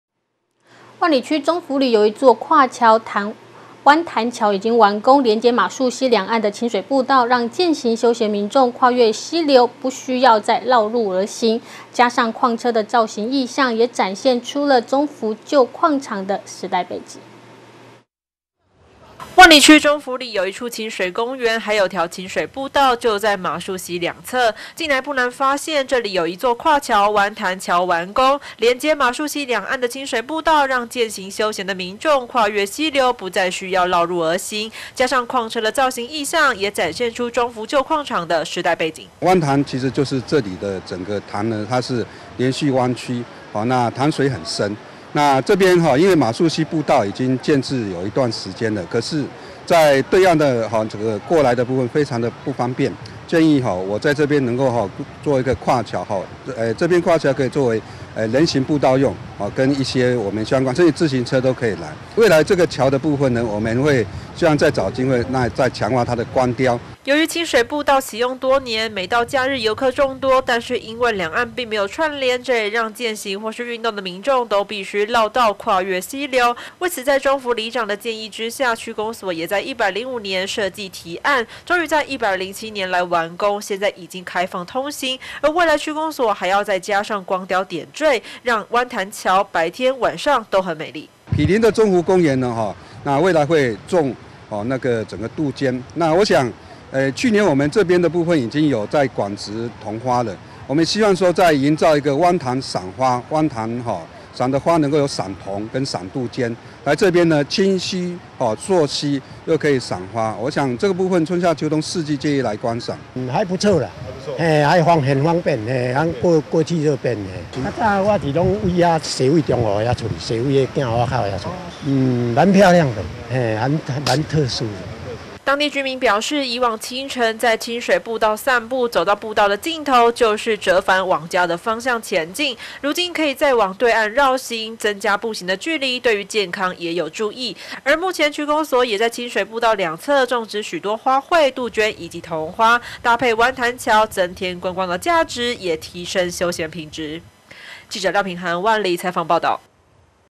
1070402新北新聞網07 萬里區中幅灣潭橋完工 礦車造型便民又美觀
萬里區中幅里有一座跨橋灣潭橋完工，連接瑪鋉溪兩岸的親水步道，讓健行休閒民眾跨越溪流不再需要繞路而行，加上礦車的造形意象，也展現出中幅舊礦場的時代背景。